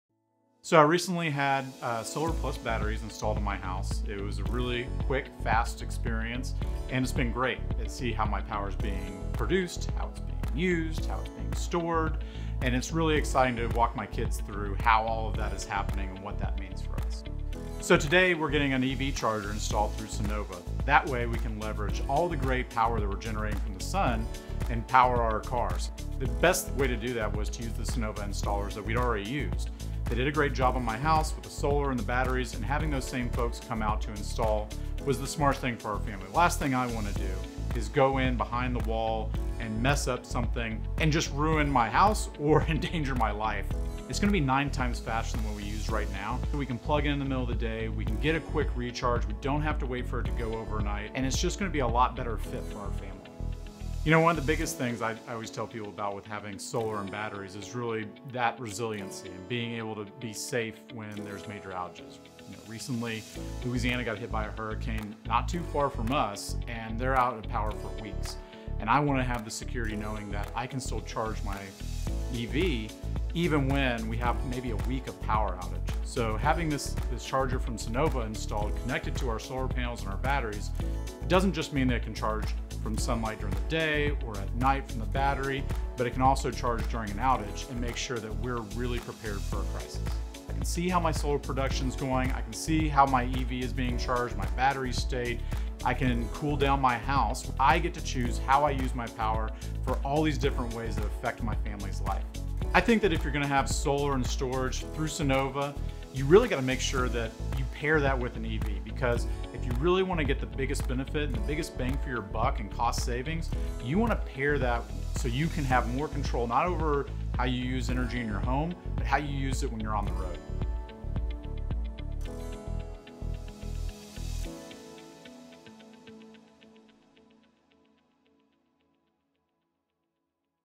Kevin Doffing Gets a New EV Charger | Sunnova
Kevin, already a solar and battery customer, is excited to have an electric vehicle charger professionally installed by a Sunnova-certified dealer so he can charge his car up to 9X faster using the power of the sun.